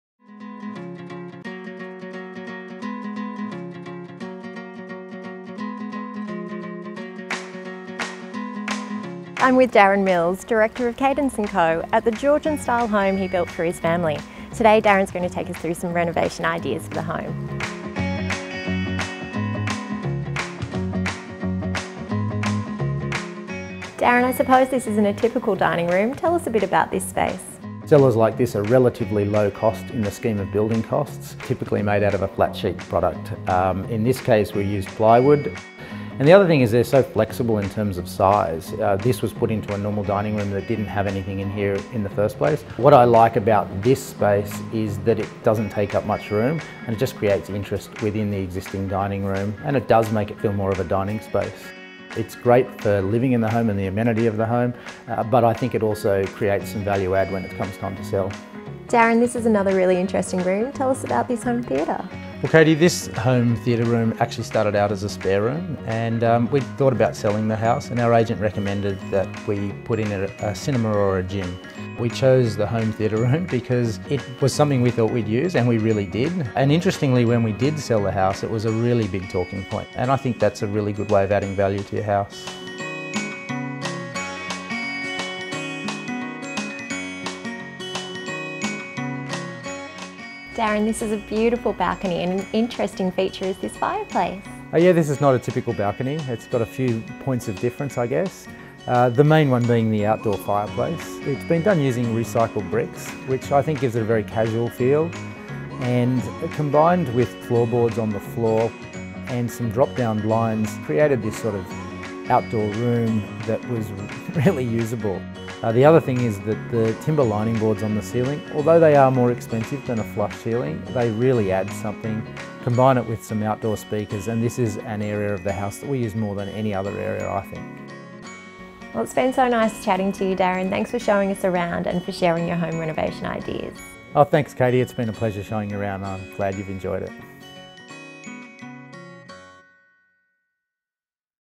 How to add value to your home
Darren Mills from Cadence and Co. shows us what he did to add value to his family home.